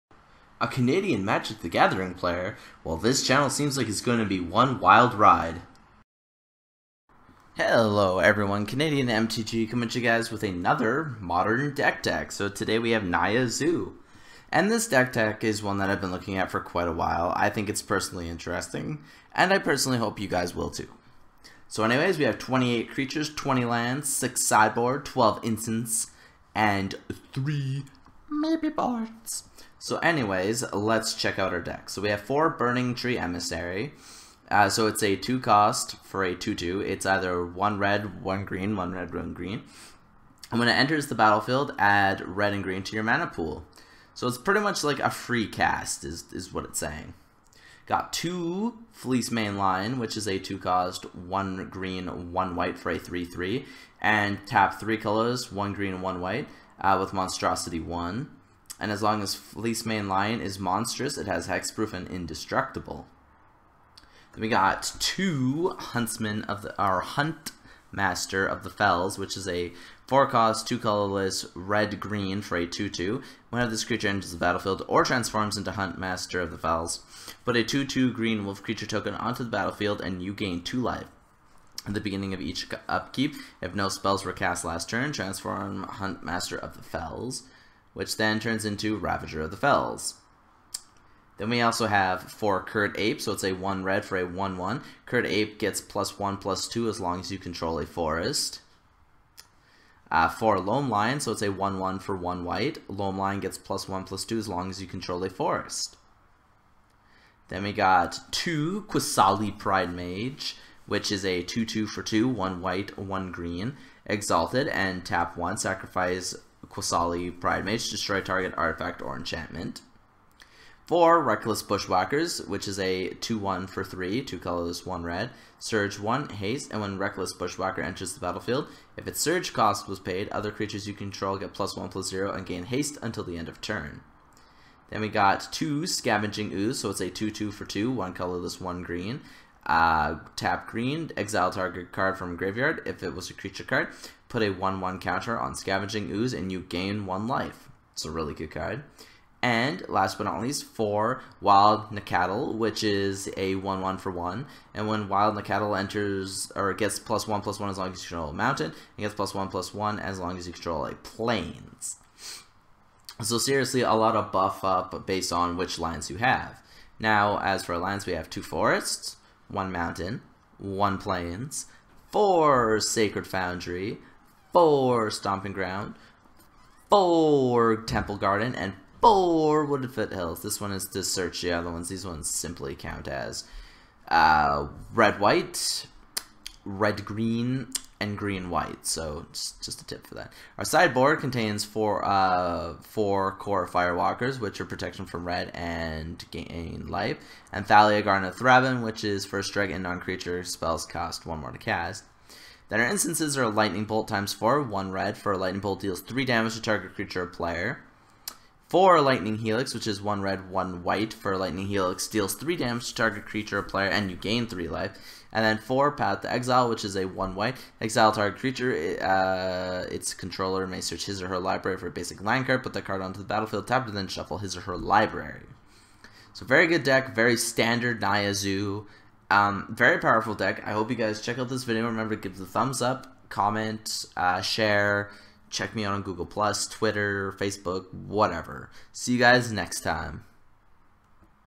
MTG Modern Deck Tech- Naya Zoo
This is one of the more expensive modern decks to build. It is not only fun to play but also very competitive.

Add me on PS4 at RatchetNBooty
Facebook me at Kelly Gillies